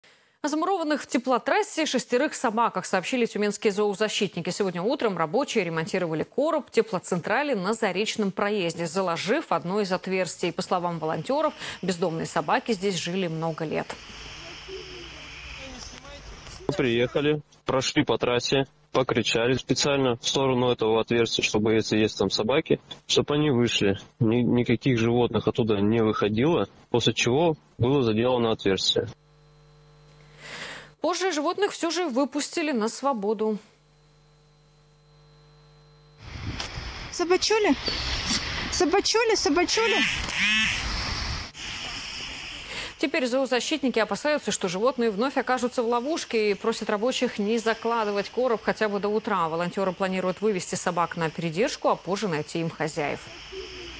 Замуровали собак в теплотрассе / Тюмень
О замурованных в теплотрассе шестерых собаках сообщили тюменские зоозащитники. Сегодня утром рабочие ремонтировали короб теплоцентрали на Заречном проезде, заложив одно из отверстий. Что дальше?